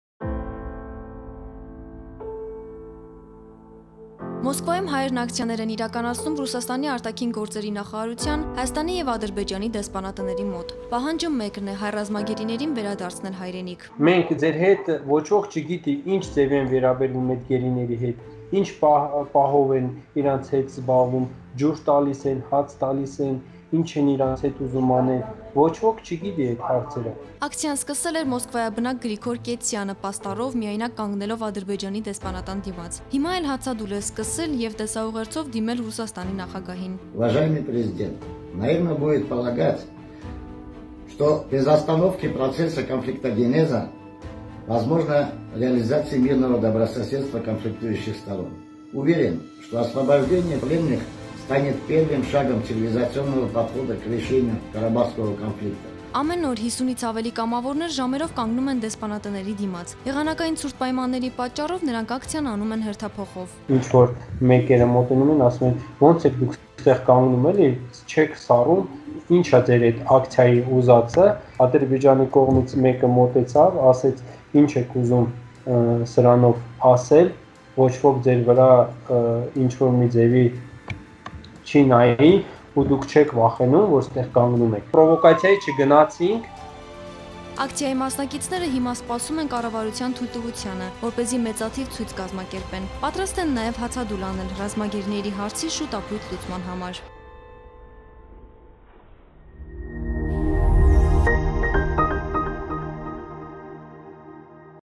The Moscow-living Armenians are holding protests demanding the return of the prisoners of war
The Moscow-living Armenians are holding protests demanding the return of the Armenian prisoners of war.